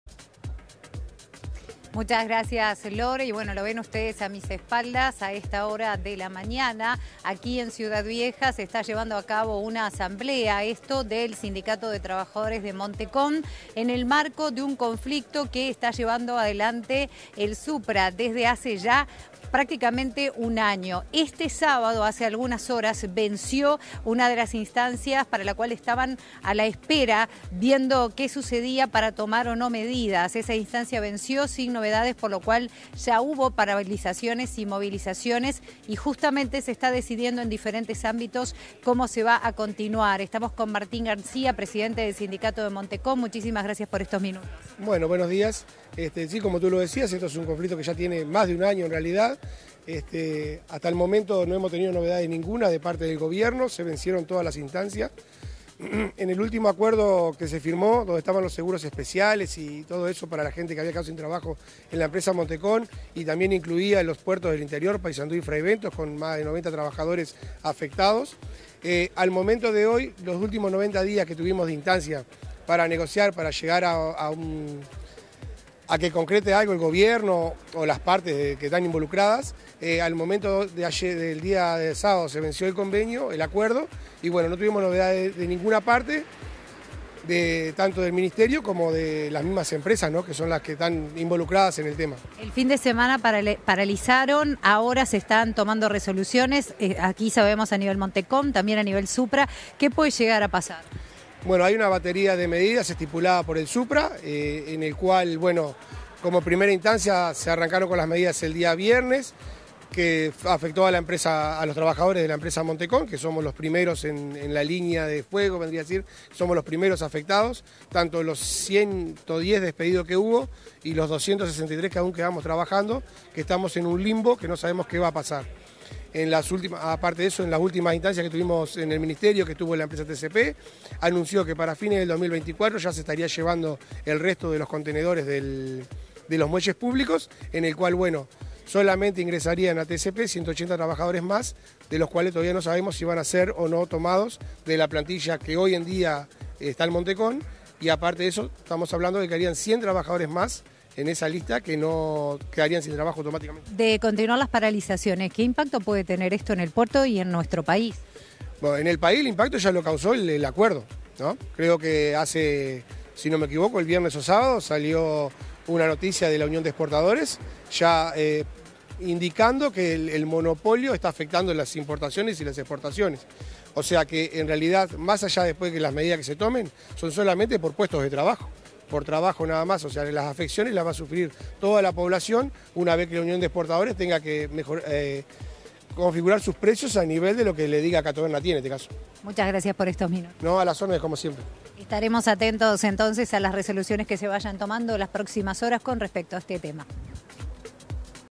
Móvil - Hay corte por tiempo indeterminado en Montecon
Están reunidos en asamblea por situación en el Puerto de Montevideo. Hay corte por tiempo indeterminado en Montecon, hablamos del tema en nuestro móvil con el integrante del sindicato de trabajadores de la empresa, Martín García. ¡Mirálo!